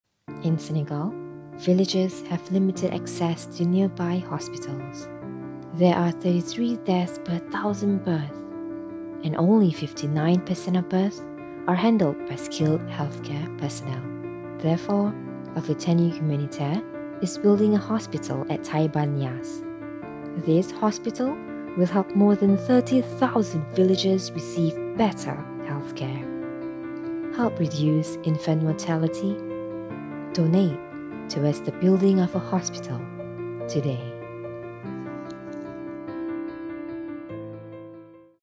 Help build a hospital in West Africa
In Senegal, villagers have limited access to nearby hospitals. There are 33 deaths per 1000 births and only 59% of births are handled by skilled healthcare personnel. Therefore, Alfityanu Humanitaire is building a hospital at Taiba Niasse. This hospital will help more than 30,000 villagers receive better healthcare.